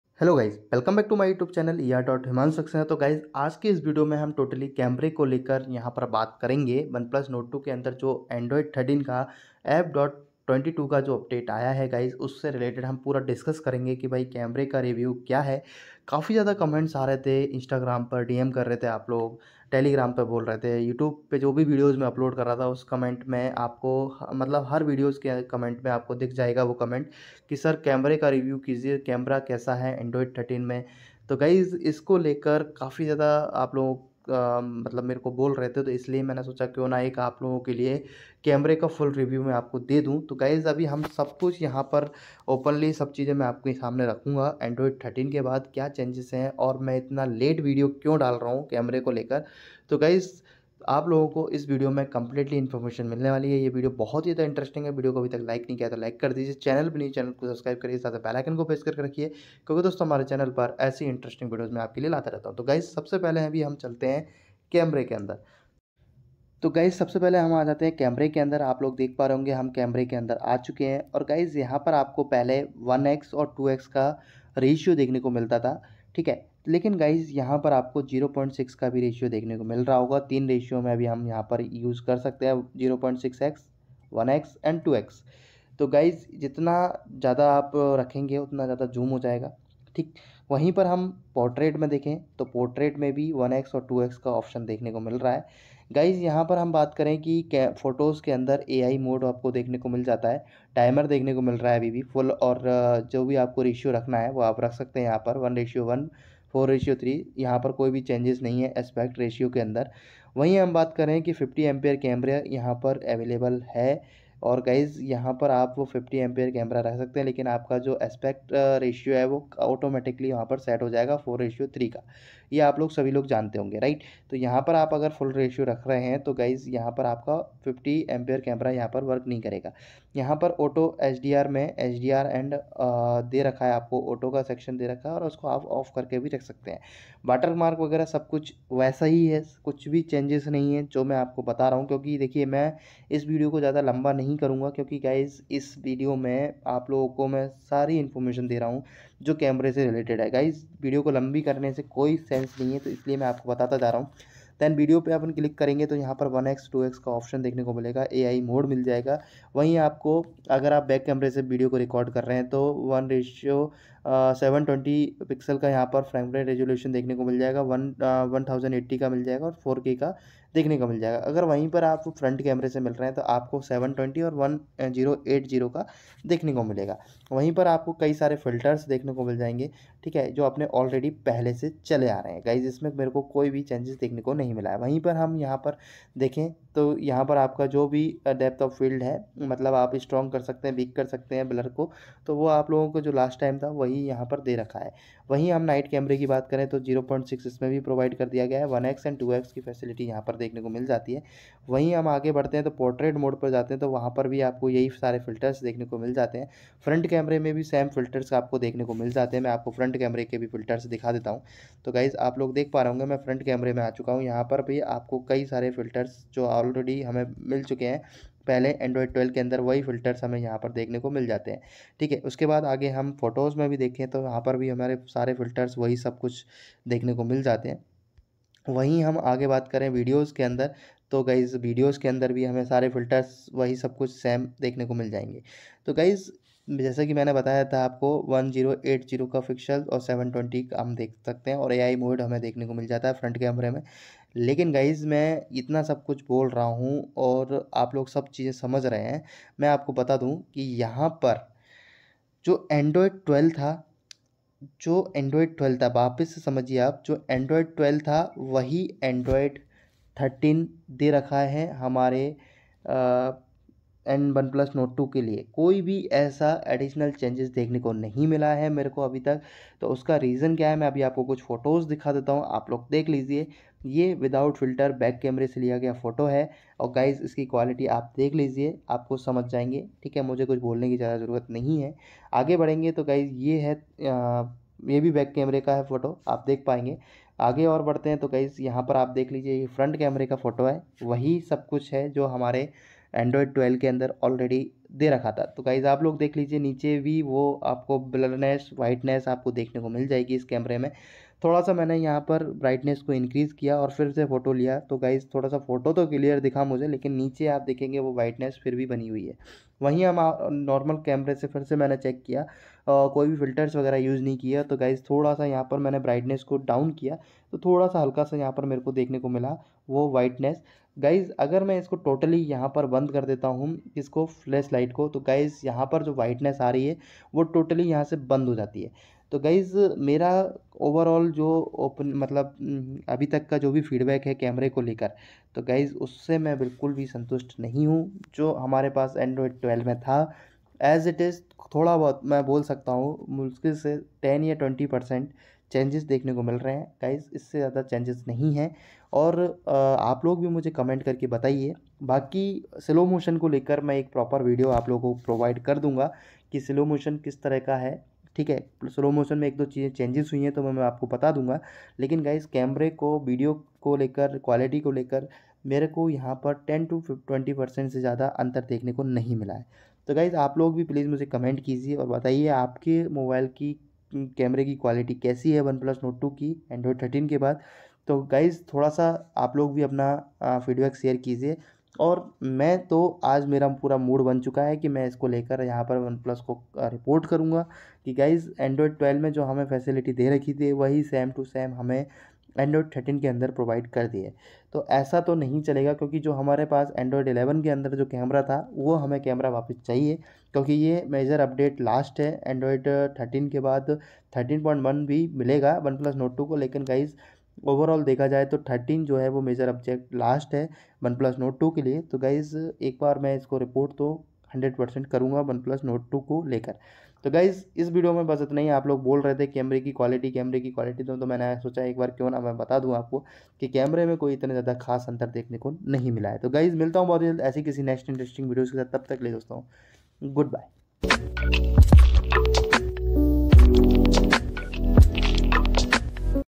After Android13 Stable Update Camera Full Review Nord2|Camera Quality Kesi Hai After Android13 Nord2
OnePlus Band Screen Guard